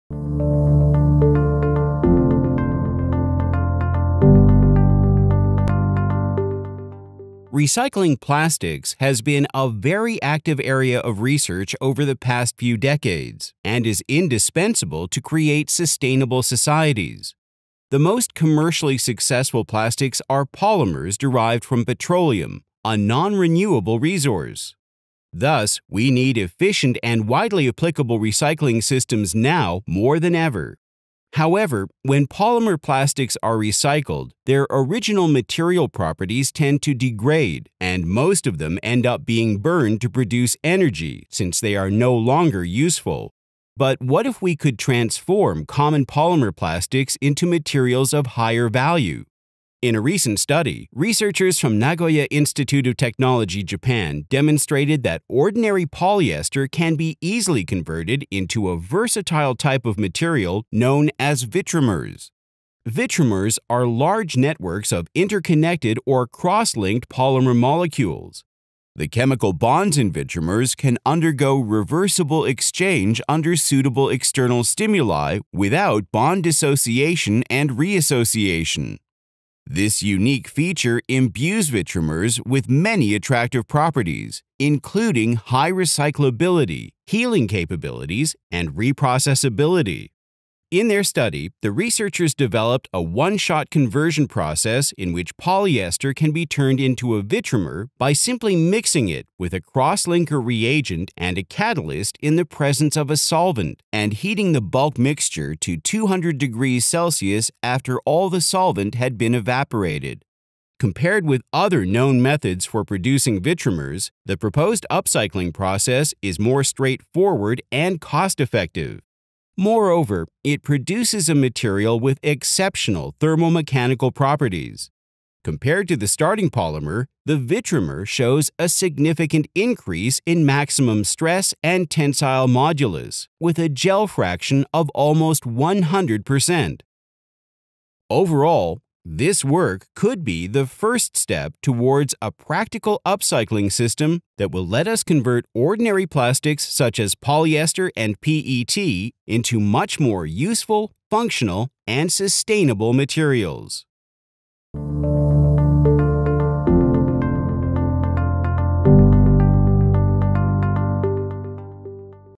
【Research】Recycling 2.0: Converting Ordinary Plastics into High-Value Vitrimers
Assistant Prof. Mikihiro Hayashi and his group from Nagoya Institute of Technology have developed a process to transform ordinary polyesters—petroleum-based polymeric plastic—into highly recyclable cross-linked materials called “vitrimers,” which maintain the recyclability and show enhanced mechanical properties. this work could be the first step towards a practical upcycling system that will let us convert ordinary plastics such as polyester and PET into much more useful, functional and sustainable materials.
Department of Engineering（Life Science and Applied Chemistry）,
Graduate School of Engineering, Nagoya Institute of Technology
Assistant Professor Mikihiro Hayashi

「リサイクル 2.0: 通常のプラスチックを高価値のビトリマーに変換」
林幹大助教らは、通常のポリエステル（石油ベースの高分子プラスチック）を、リサイクル性を維持し、機械的特性を向上させる「ビトリマー」と呼ばれるリサイクル性の高い架橋材料に変換するプロセスを開発した。 本成果は、通常のプラスチックをより有用で機能的かつ持続可能な素材に変換できる実用的なアップサイクル システムへの第一歩となる可能性を示唆している。
名古屋工業大学大学院工学研究科工学専攻（生命・応用化学領域）
林幹大　助教